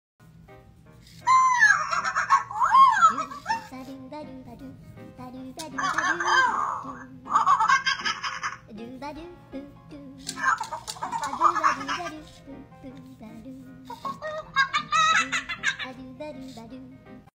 FUZZY Fingerlings love to be pet!!🐵💜
Summer 2023 is going to be BANANAS!🍌 Here’s what you need to know about ALL-NEW Fingerlings

🍌 Heart-to-heart connection! They know when they’re on your finger

🍌 Posable tails for more ways to play and display

🍌 New Soft FUZZY hair

🍌 4 baby monkeys with unique personalities & 70+ reactions